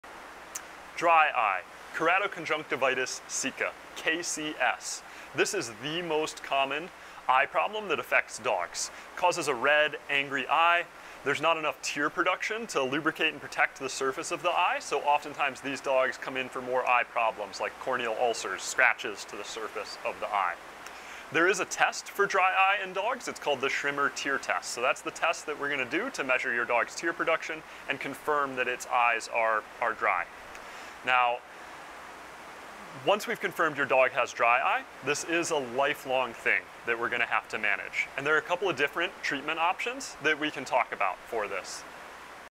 Dry Eye, KCS, Keratoconjunctivitis Sicca
Not enough tear production. Red, angry eye.

Transcript:
Dry eye. Keratoconjunctivitis sicca. KCS. This is the most common eye problem that affects dogs--causes a red angry eye. There's not enough tear production to lubricate and protect the surface of the eye. So, oftentimes, these dogs come in for more eye problems, like corneal ulcers--scratches to the surface of the eye. There is a test for dry eye in dogs; it's called the Schirmer tear test. So that's the test that we're going to do to measure your dog's tear production and confirm that its eyes are are dry. Now once we've confirmed your dog has dry eye, this is a lifelong thing that we're going to have to manage, and there are a couple of different treatment options that we can talk about for this.